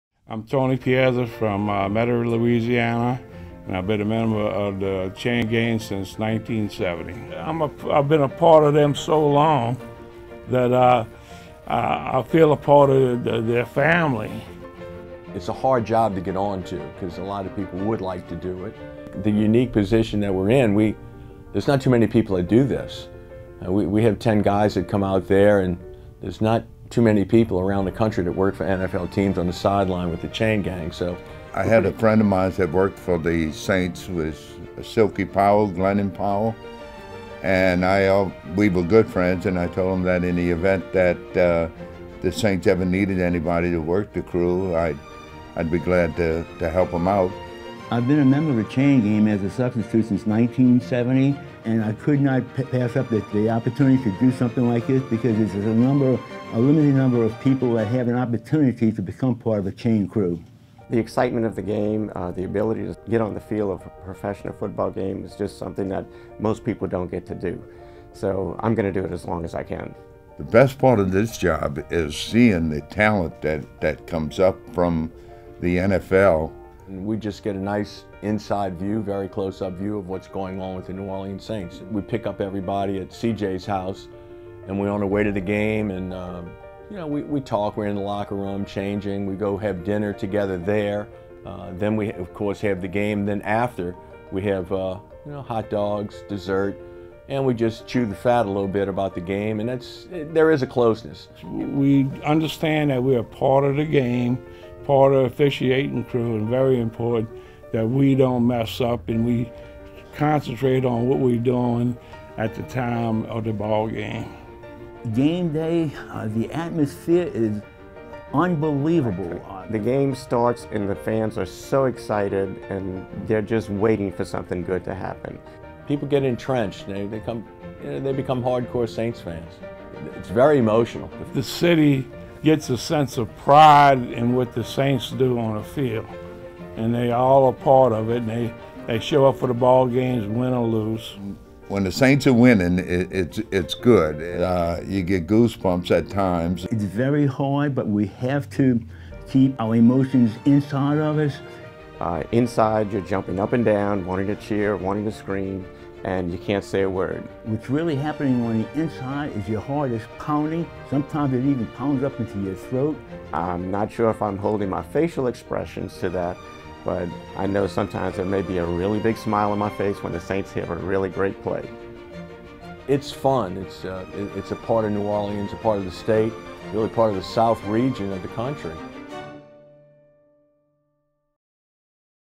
LoveTheSaints: The Chain Gang
Watch the exclusive group of chain crew members talk about being on the sidelines at New Orleans Saints games and why they